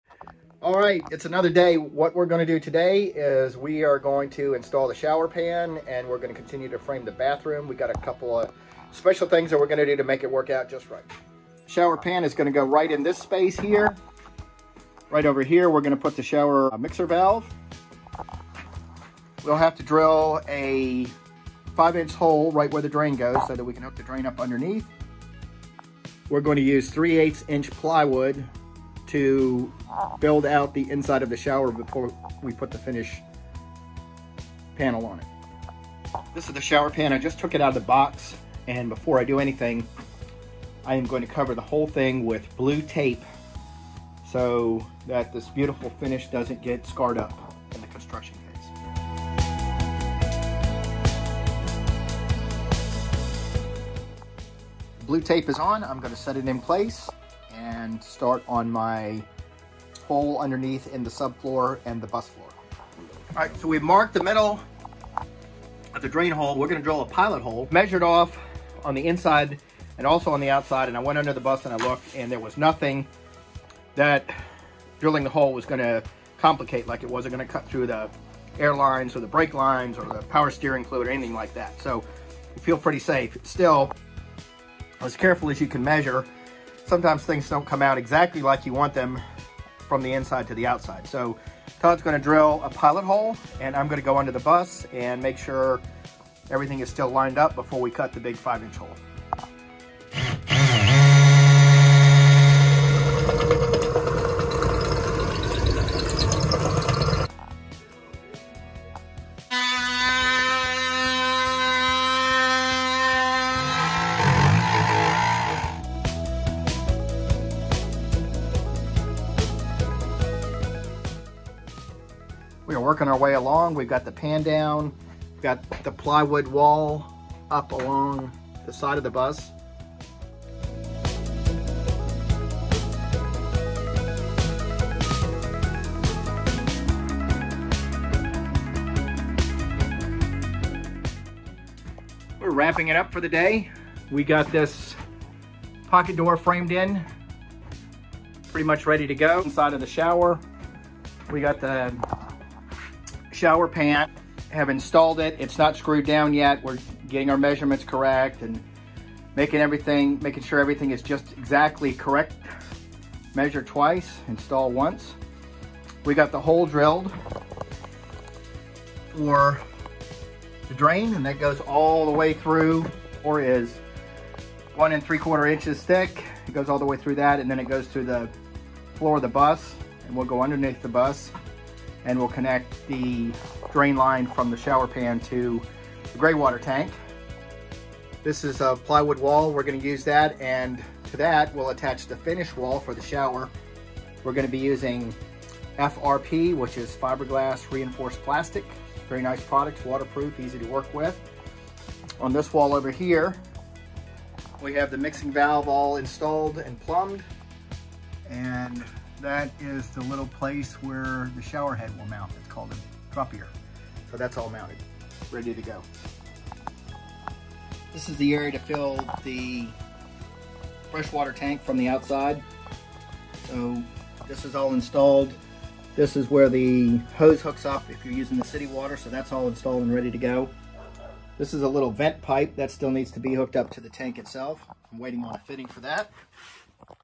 Installing shower pan, mixing valve for shower. Skoolie converting school bus to tiny home
In this episode we cut a big 5 inch hole in the floor to install a shower pan. Plumbing moves along with mixing valve and shower plumbing rough in’s.